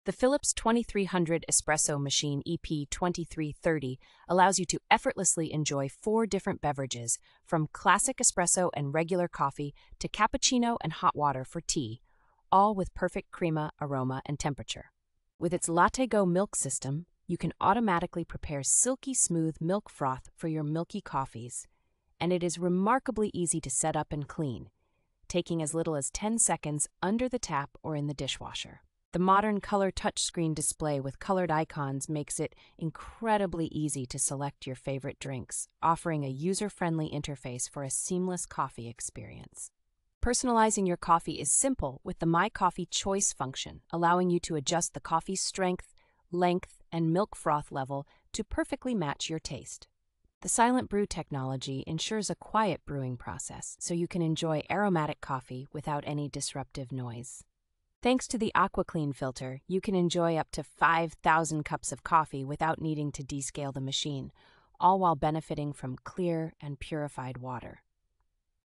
REVIEW (2024): PHILIPS 2300 Espresso Machine EP2330. ESSENTIAL details.
The PHILIPS 2300 Espresso Machine EP2330 allows you to effortlessly enjoy four different beverages, from classic espresso and regular coffee to cappuccino and hot water for tea, all with perfect crema, aroma, and temperature.

With its LatteGo milk system, you can automatically prepare silky smooth milk froth for your milky coffees, and it is remarkably easy to set up and clean, taking as little as ten seconds under the tap or in the dishwasher.

The modern color touch screen display with colored icons makes it incredibly easy to select your favorite drinks, offering a user-friendly interface for a seamless coffee experience.

Personalizing your coffee is simple with the My Coffee Choice function, allowing you to adjust the coffee strength, length, and milk froth level to perfectly match your taste.

The SilentBrew technology ensures a quiet brewing process, so you can enjoy aromatic coffee without any disruptive noise.

Thanks to the AquaClean Filter, you can enjoy up to five thousand cups of coffee without needing to descale the machine, all while benefiting from clear and purified water.